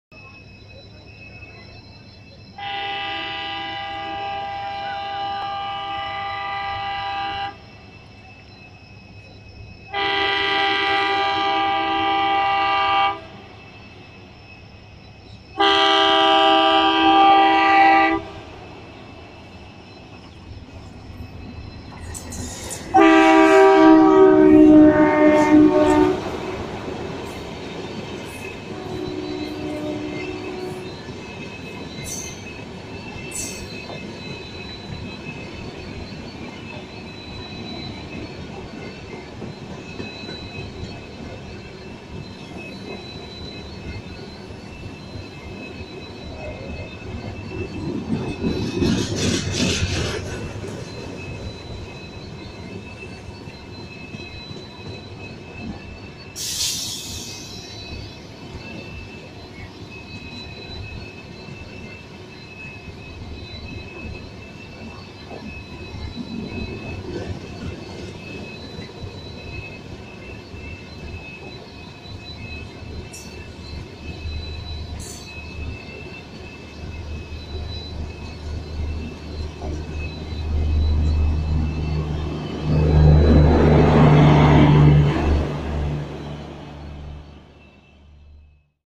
Single Line Crossing Trains | Pooja SF Express , Ala Hazrat Express | Indian Railway
Single Line Crossing in Indian Railway - ALCo vs EMD
Trains - Ajmer - Jammu Tawi Pooja SF Express behind WDM3A
              Bareilly - Bhuj Ala Hazrat Express behind WDP4D

Location - Malakhera (Jaipur - Alwar Route)